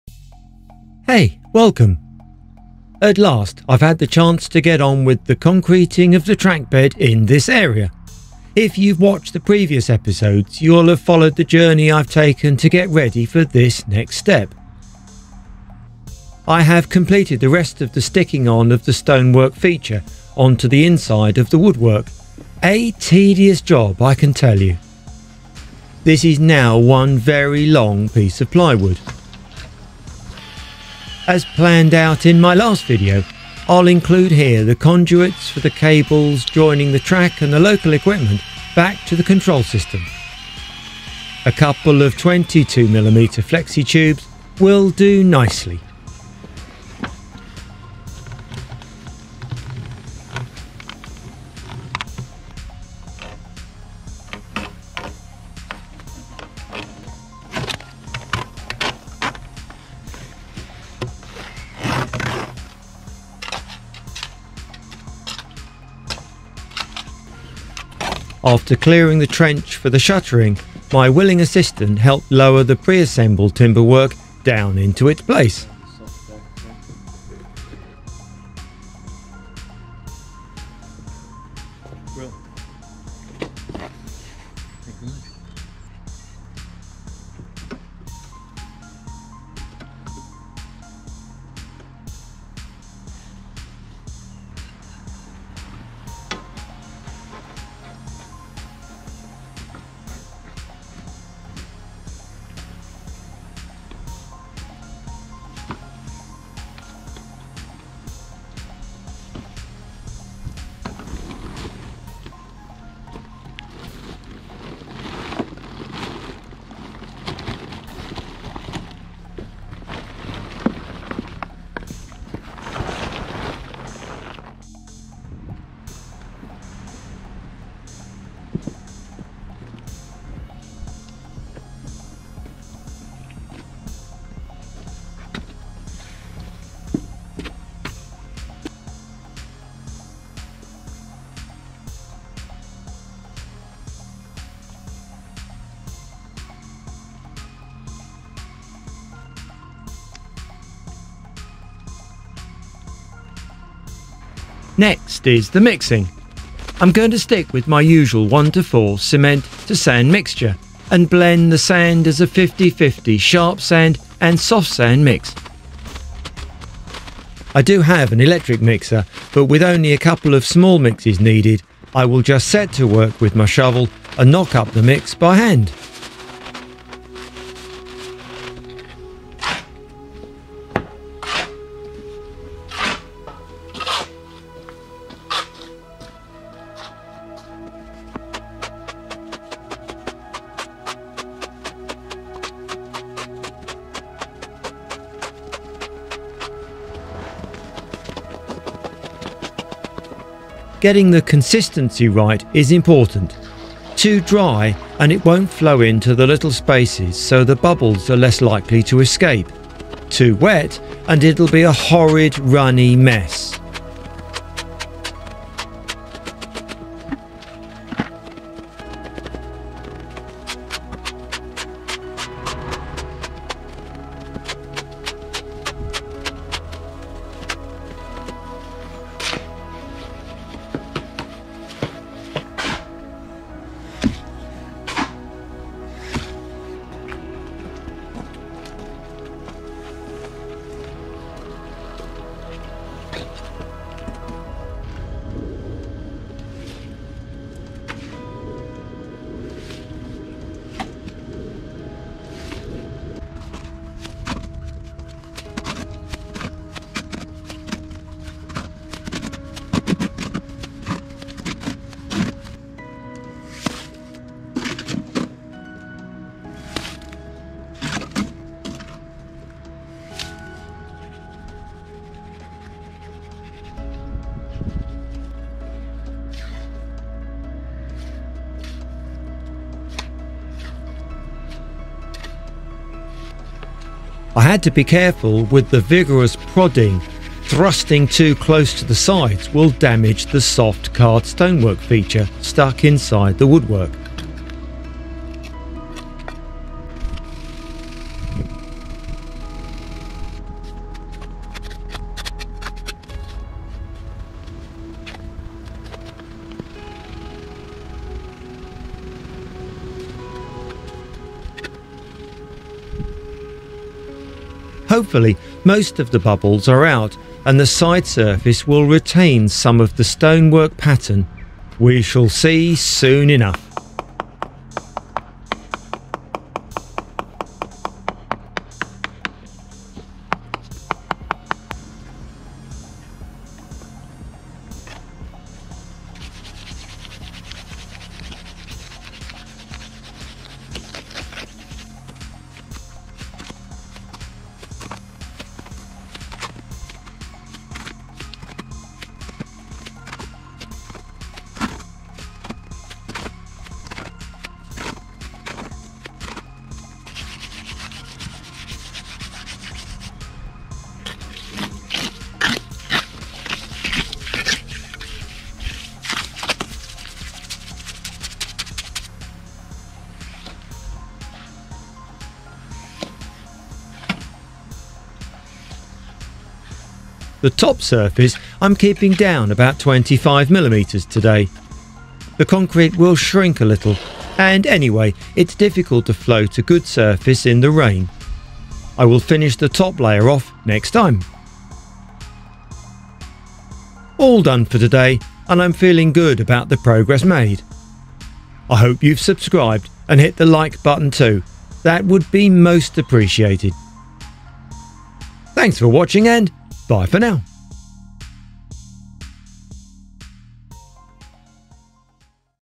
Messing about with concrete | Linleys Garden Railway Developments Autumn 2020 Part 7
This seventh short update takes a big bold step with the laying of the concrete to form the trackbed and embankment on one side of the path bridge. It has taken some weeks to get here but, at last, the mix is done and concrete is poured. Some prodding and thrusting was needed to remove air bubbles and, hopefully, get a good stone-block effect on the sides of the embankment wall.  We shall see soon enough.

If you have any questions, please post them in the comments below.

Warren

Video Production notes:
- Photos and video captured by GoPro Hero 8 Black
- Video produced using FilmoraX  – just upgraded from version 9
- Filmed on location at Linleys Garden Railway
- All pets and family members were kept safe during the trackbed construction work and video recording.